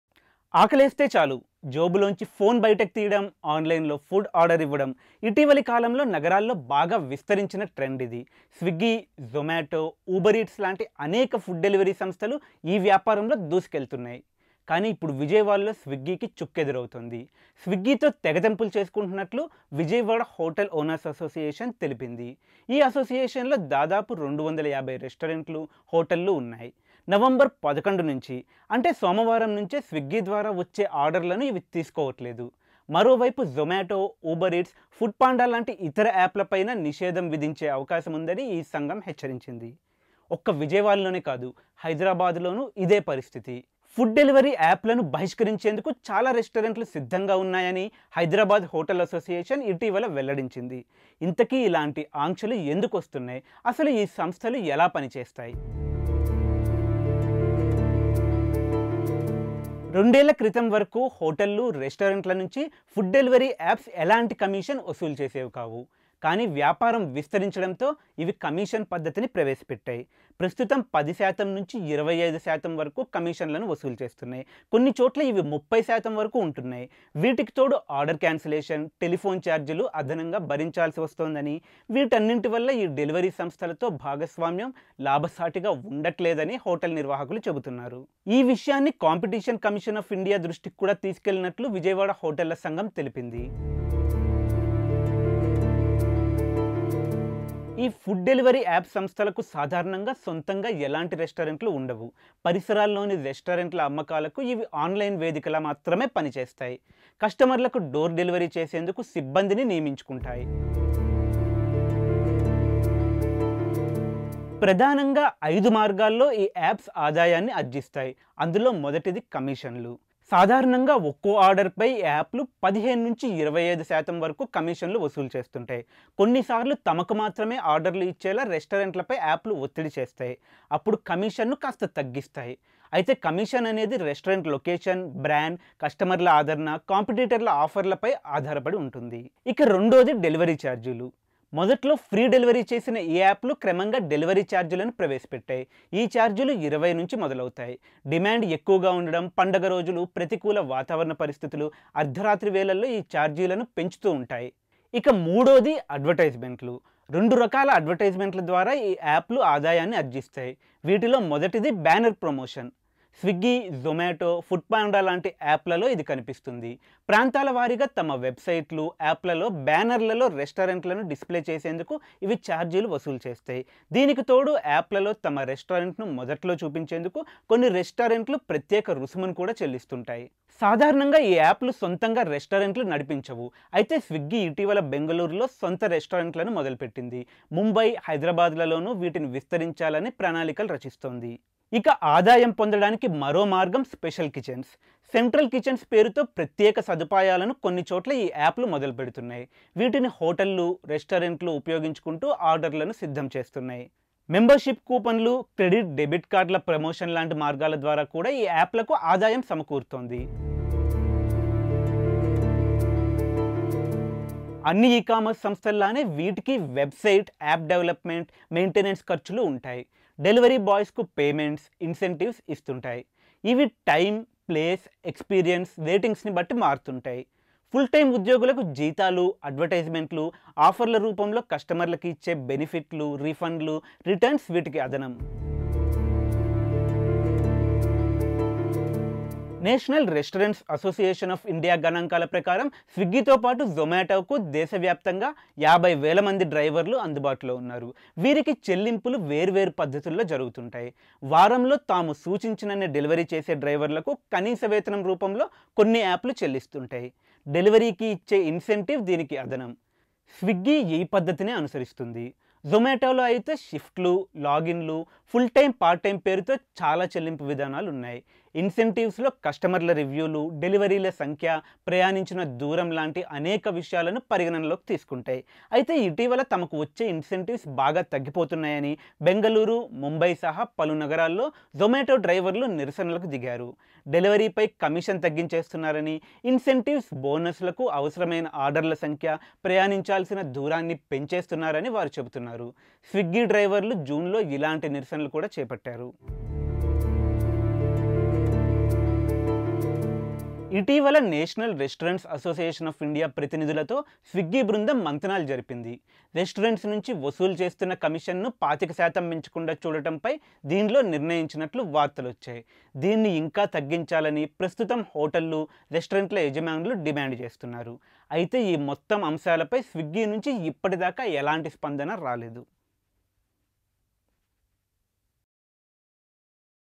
Swiggyని విజయవాడలో హోటళ్లు Ban చేయాల్సిన అవసరం ఎందుకొచ్చింది? ఈ యాప్స్ కు డబ్బులు ఎలా వస్తాయి?
Swiggyని విజయవాడలో కొన్ని హోటళ్లు ఎందుకు నిషేధించాయి? హైదరాబాద్‌లోనూ ఇలాంటి యాప్స్‌ను బహిష్కరిస్తామని కొన్ని హోటళ్లు ఎందుకంటున్నాయి? అసలు ఫుడ్‌ డెలివరీ యాప్స్‌కు ఆదాయం ఎలా వస్తుంది? డ్రైవర్లకు అవి జీతాలు ఎలా చెల్లిస్తాయి?